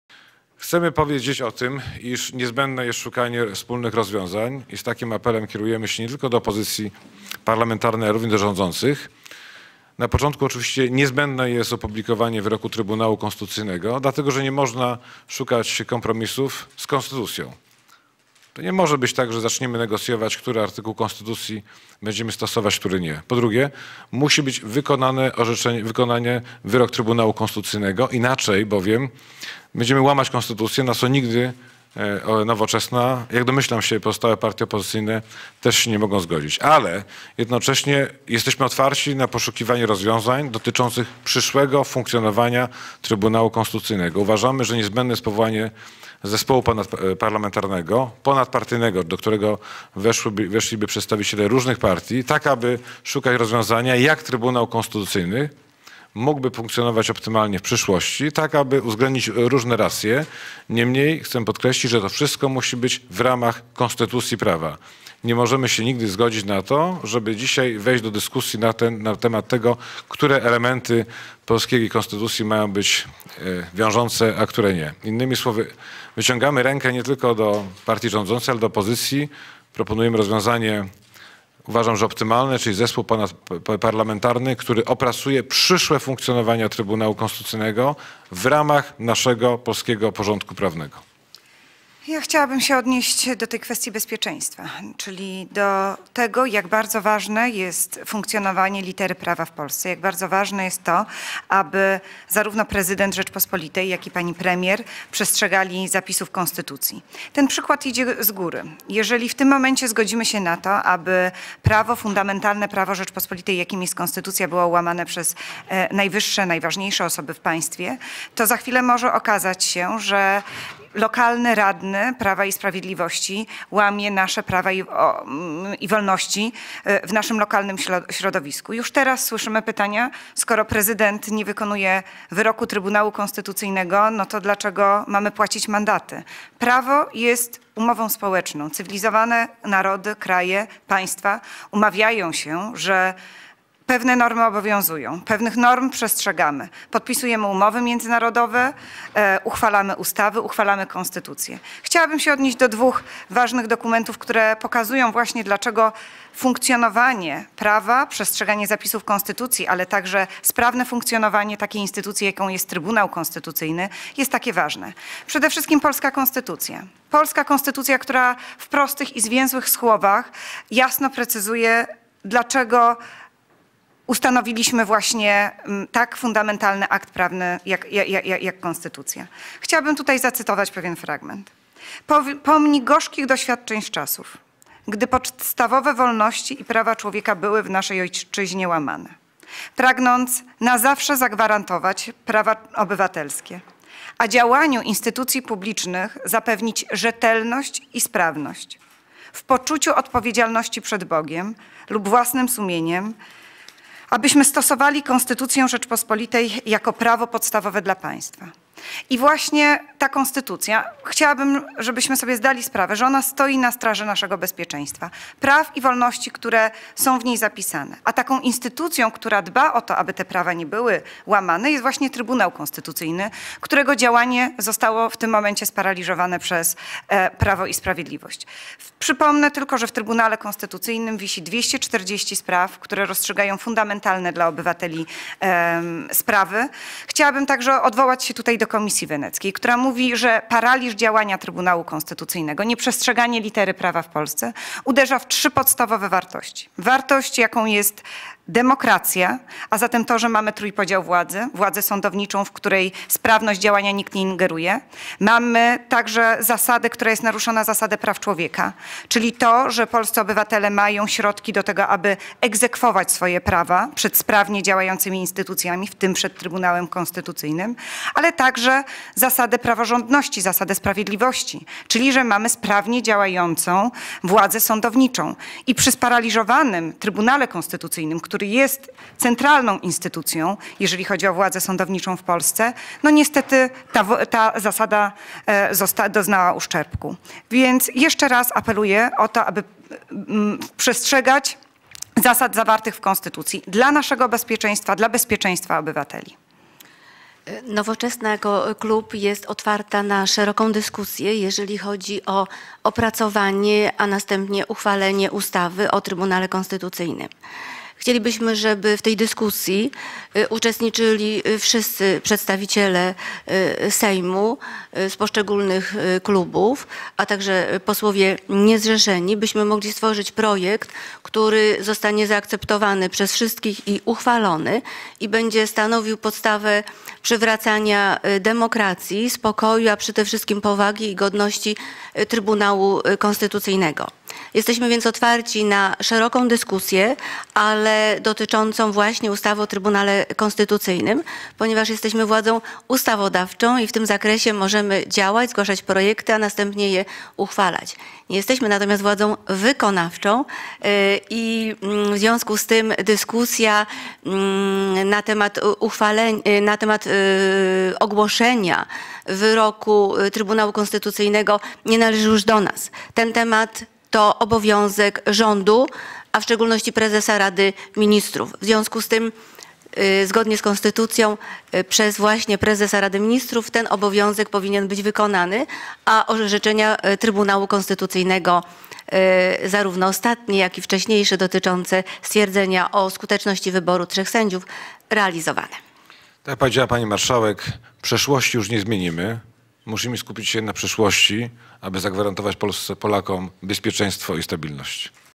R. Petru, K. Gasiuk-Pihowicz, B. Dolniak o kryzysie wokół TK, 16.03.2016
Rozwiązanie konfliktu wokół Trybunału Konstytucyjnego jest możliwe